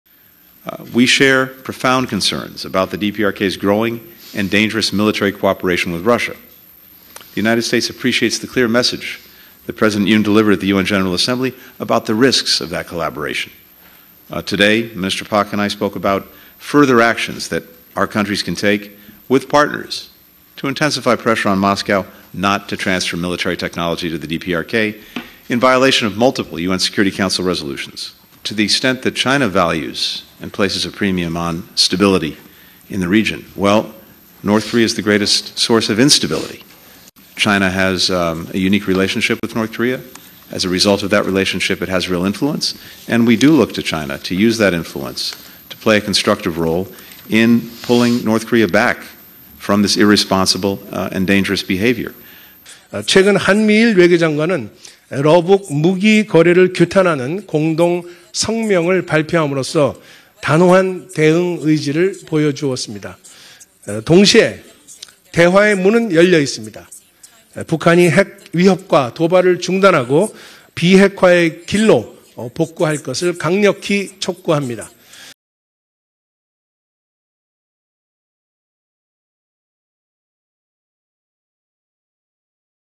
Blinken warns of 'growing and dangerous' North Korea-Russia military ties
US Secretary of State Antony Blinken warns of the increasing military cooperation between North Korea and Russia, as he visits key ally South Korea. I 🎥:  Jung Yeon-Je, Richard A. Brooks / AFP / DC Pool